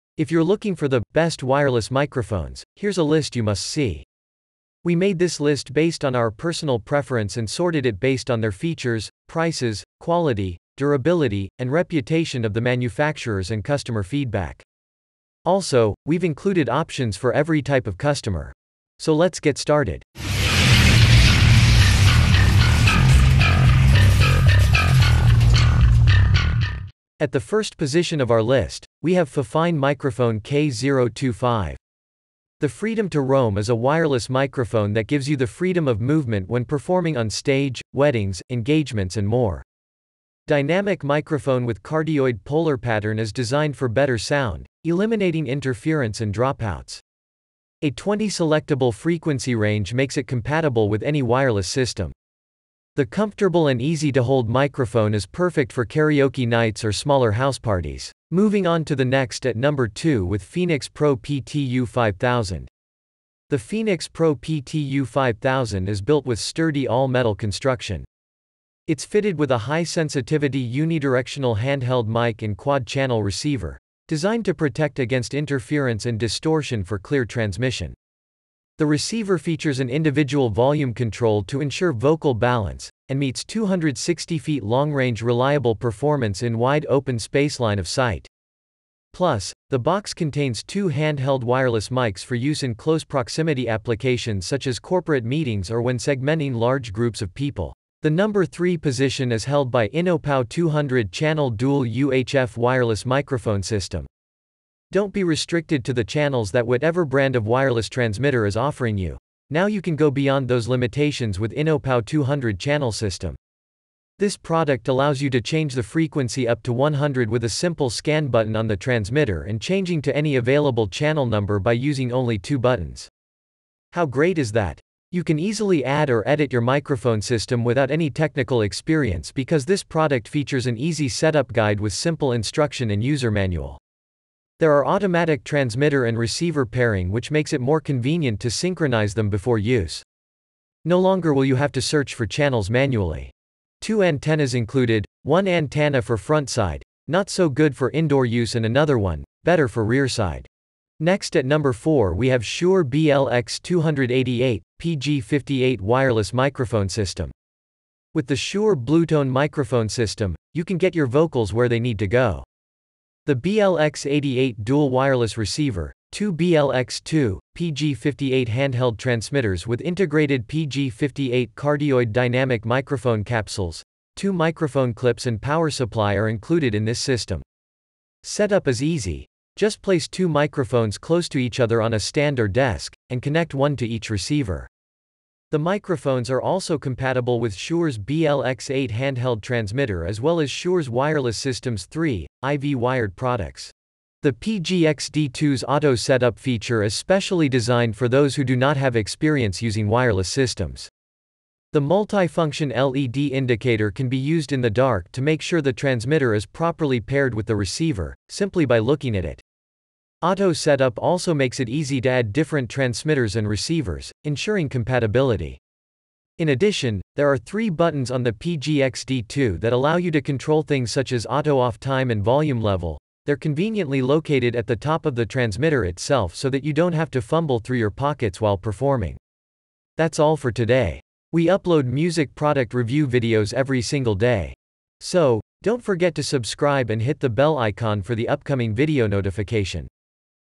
Wireless Microphones: Top Rated Microphone On Amazon
You Can Get Best Wireless Microphones From Here:

1 . Fifine Microphone K025

2 . Phenyx Pro PTU 5000

3 . innopow 200-Channel Dual UHF Wireless Microphone System

4 . Shure BLX288/PG58 Wireless Microphone System

5 . Shure PGXD24/SM58-X8 Digital Handheld Wireless System